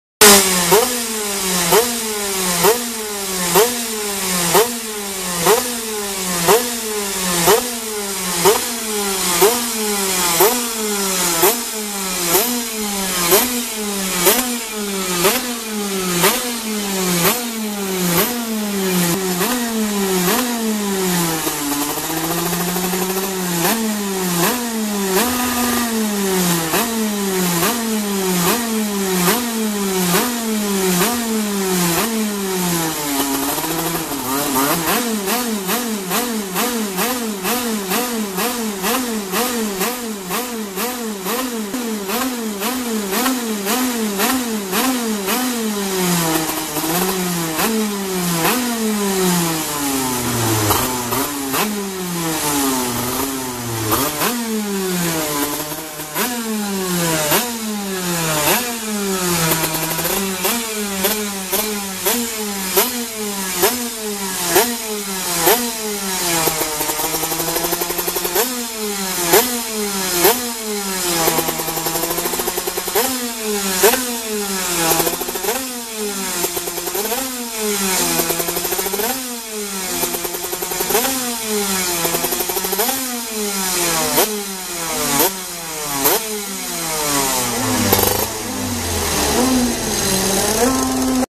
Marmita go-kart IVANORGANIZZA il Blog del Kart
La marmitta del kart e tutto quello che vuoi sapere sul mio blog
Ripropongo questo video già pubblicato da Angelo Catapano perchè molti appassionati possano apprezzare il sound veramente emozionante.